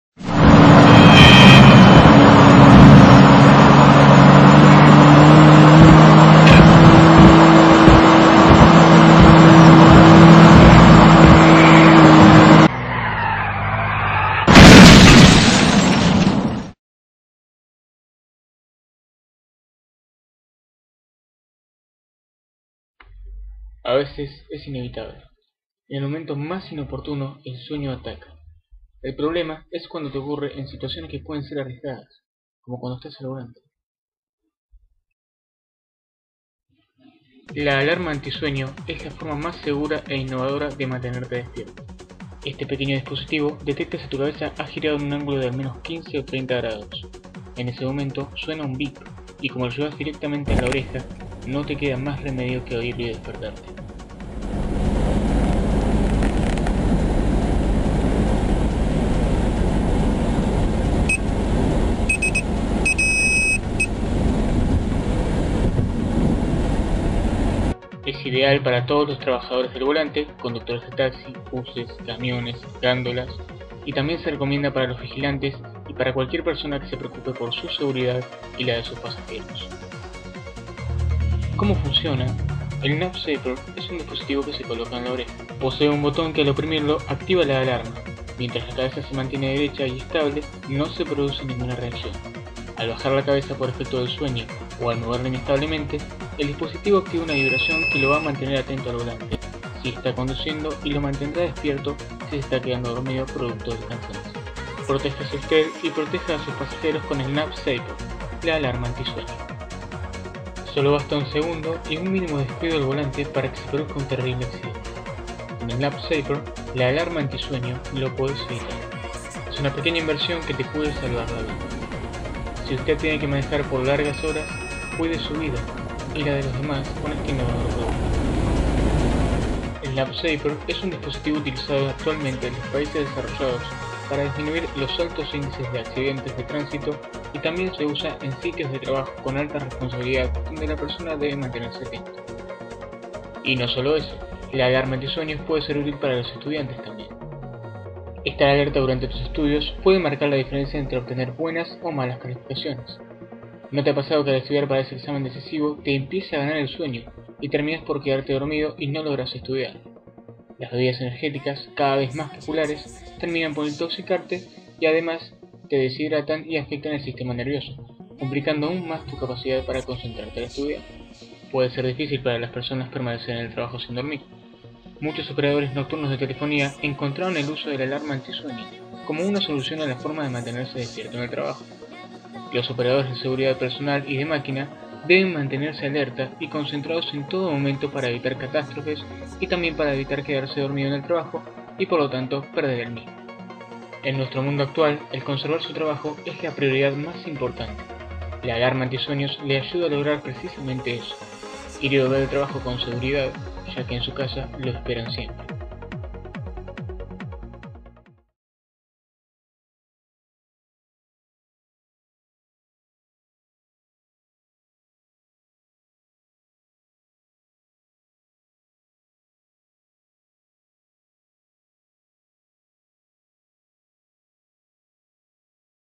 La Alarma Anti-Sueño [Para prevención de accidentes automovilísticos]
Éste es uno de mis últimos trabajos que hice para una amiga para la facultad. Habla sobre la famosa alarma anti-sueño que nos ayuda a mantenernos despiertos mientras conducimos a altas horas de la noche, se aplica ya sea para conductores comunes y corrientes, como también para taxistas, camioneros, etc. Espero les guste.